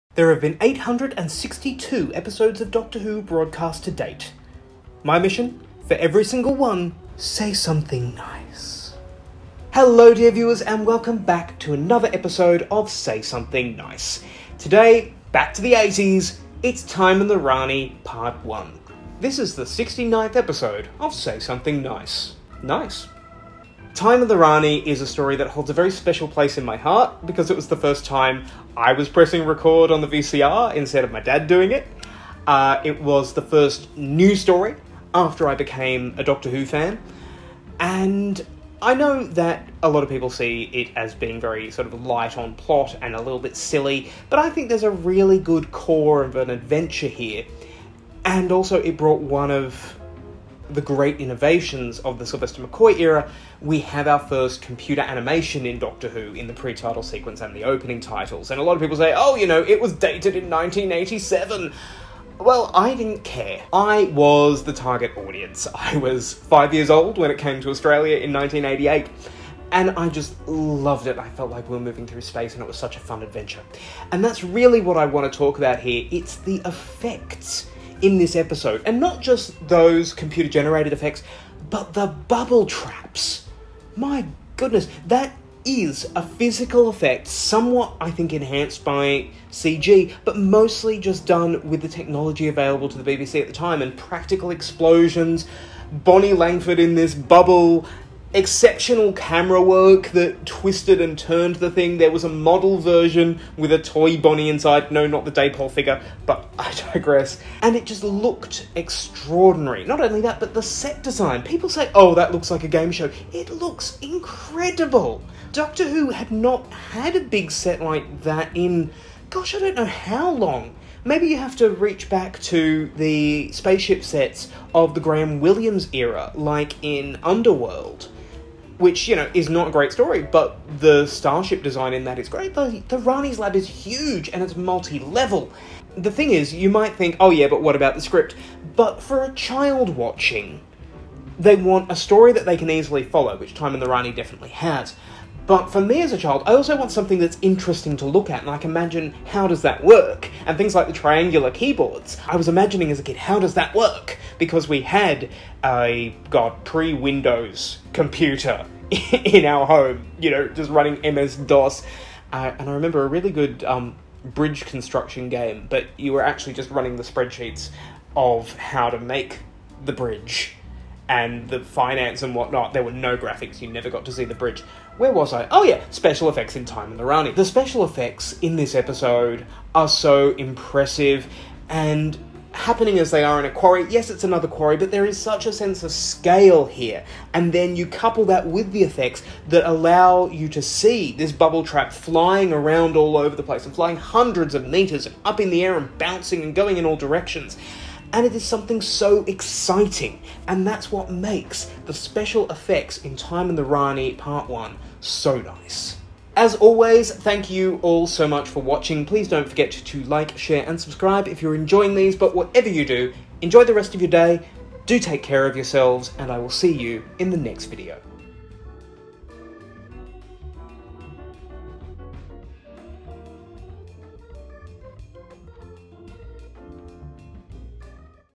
Doctor Who- Time and the Rani Part One (Say Something Nice #69)
In this very nice 69th installment, it's all about nice pink balls bouncing around rocky pits.